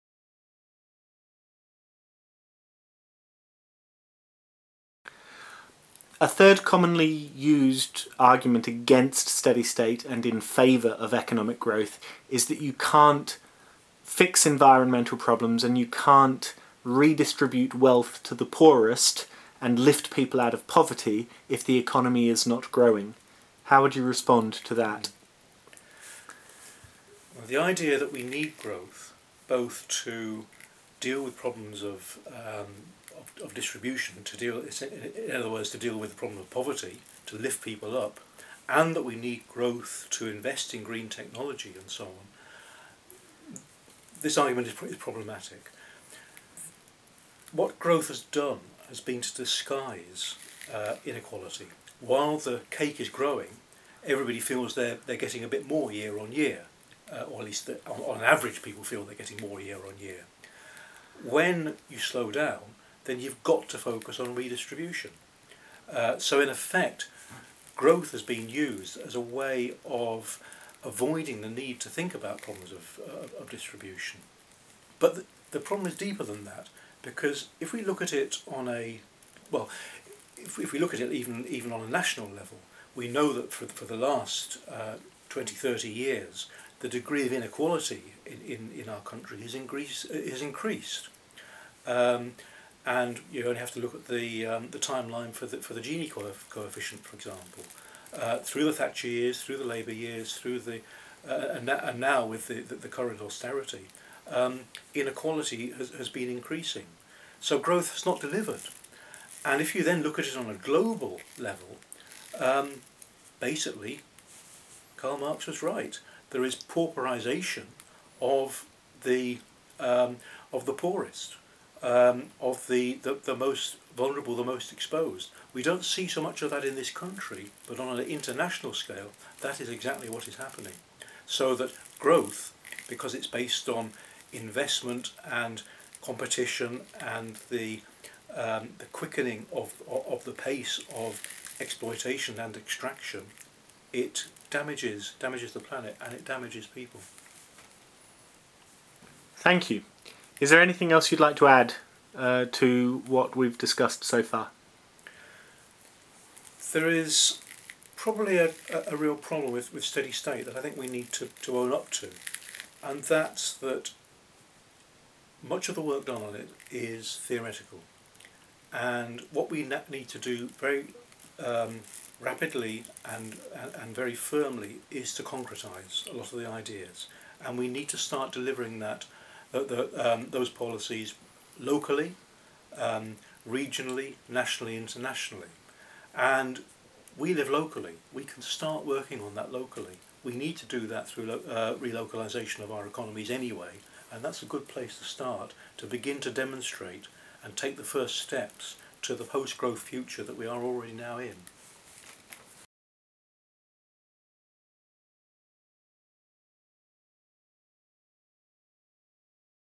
Steady State Manchester: "But we need to grow the economy to deal with poverty and eco-problems"
Short interview with Mark Burton about the argument that only through growing the economy can we deal with poverty and ecological problems